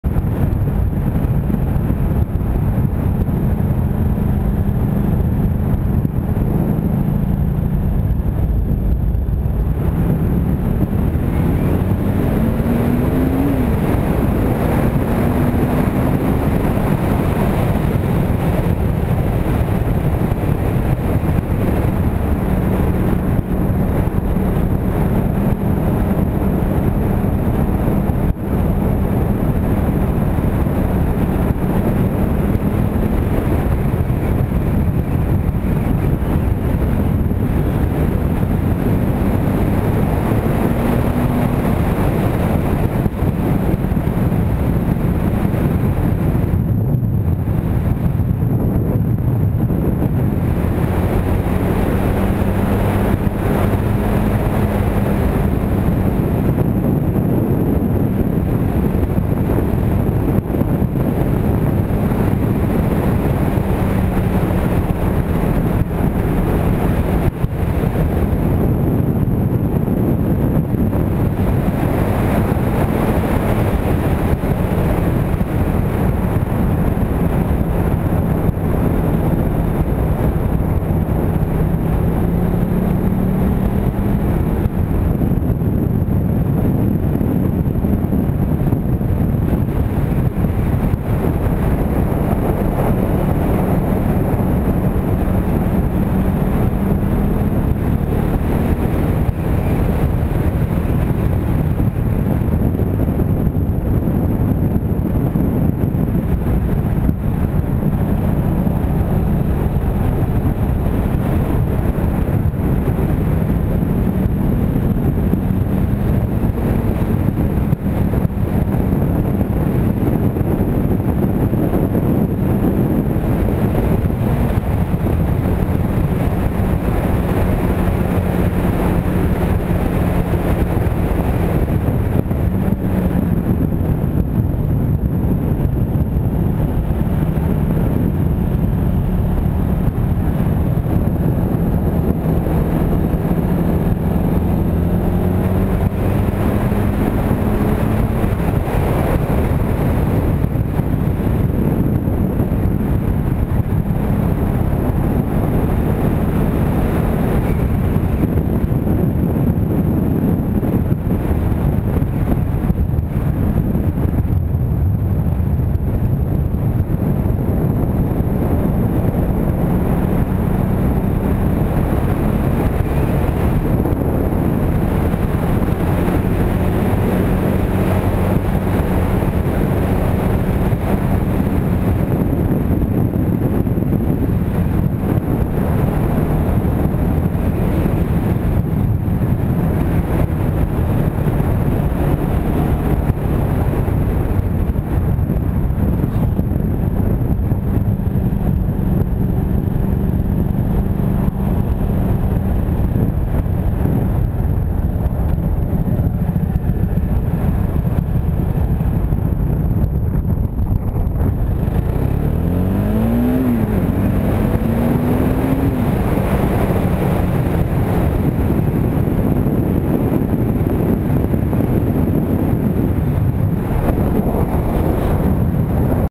Highway 9 South (San Jose) - KTM Duke 690 following Ducati Paningale
Riding highway 9 south from someplace south of Saratoga to the highway 35 intersection.

Following a Ducati 1199 Paningale by happenstance.

Riding KTM Duke 690

Filming with Polaroid XS100 720p/60fps